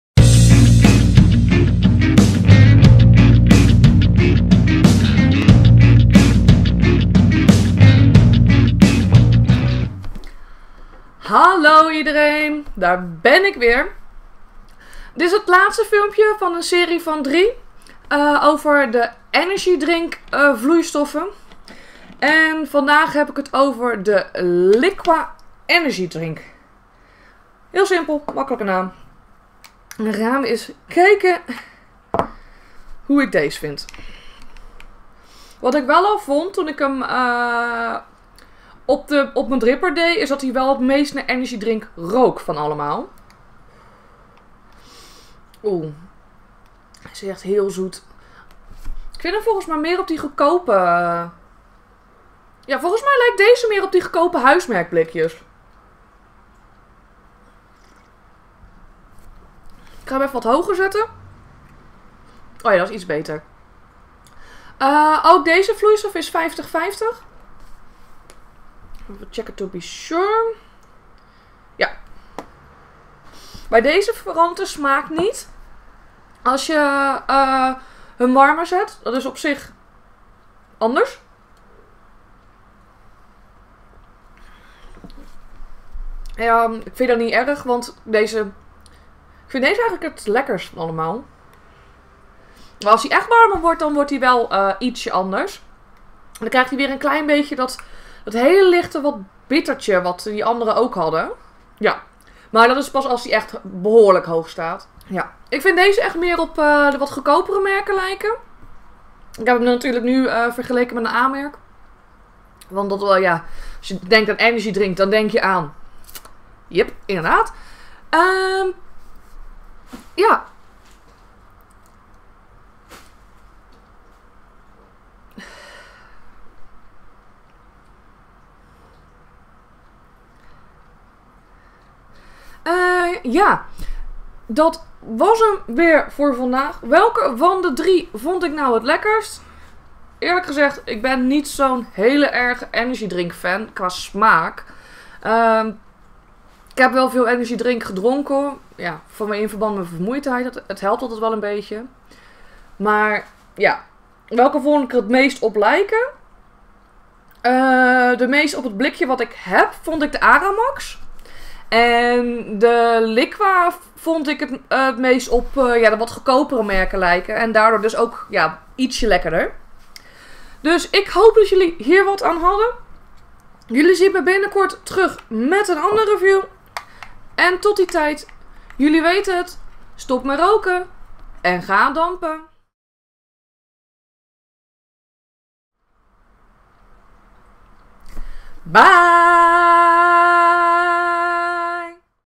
Drie keer energie. Nr3: Liqua, Energy drink
In deze serie van 3 filmpjes reviewd Loes drie verschillende e-vloeistoffen met energie drink smaak.
Dit is de laaste, nummer 3: Liqua, Energy drink